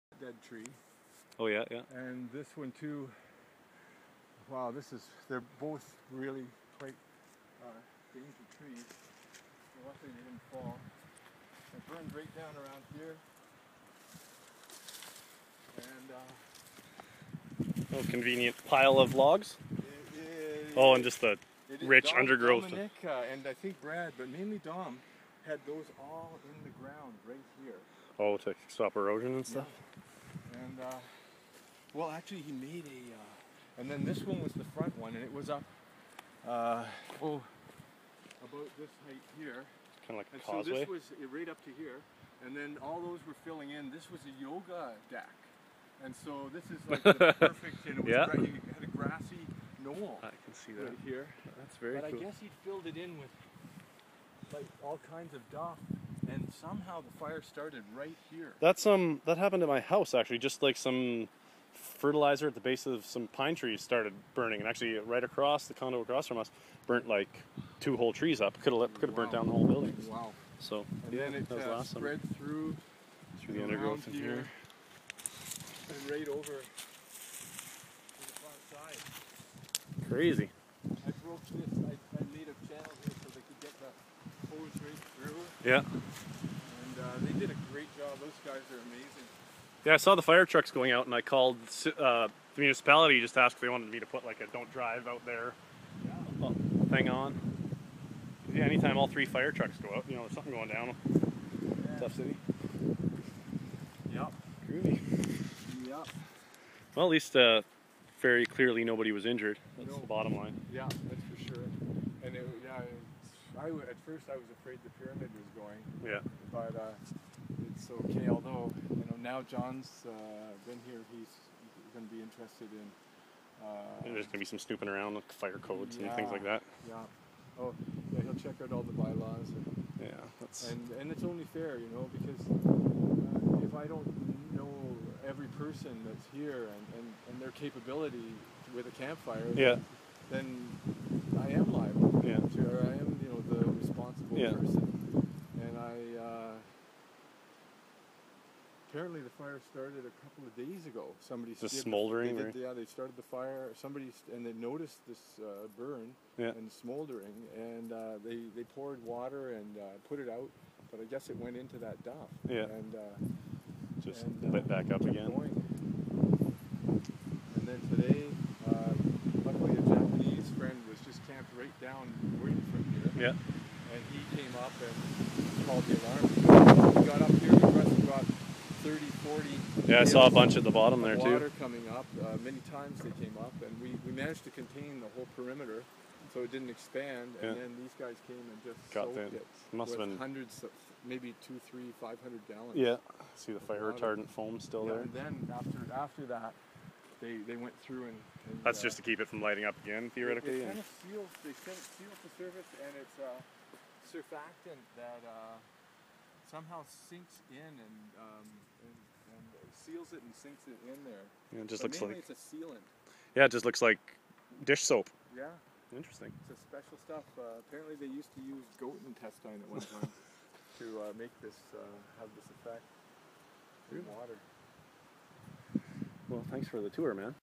Mike Poole talks us through a recent fire on the property
July 12. Video By Geoff Johnson of Long Beach Radio.  Nobody was hurt.